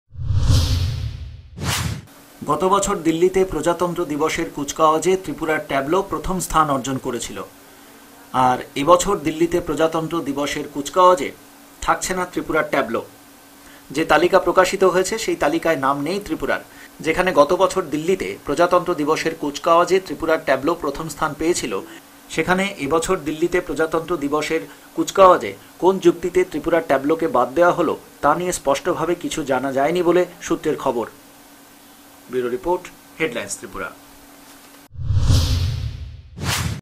প্রজাতন্ত্র দিবসে দিল্লীতে এবার স্থান পেল না ত্রিপুরার ট্যাবলো
গত বছর প্রজাতন্ত্র দিবসে দিল্লীতে ত্রিপুরার ট্যাবলো প্রথম স্থান পেয়েছিল। কিন্তু এবছর ত্রিপুরা অংশগ্রহণের সুযোগই পেল না। এবছর ত্রিপুরার সাথে কেন এরকম ব্যবহার তা বোধগম্য হচ্ছে না।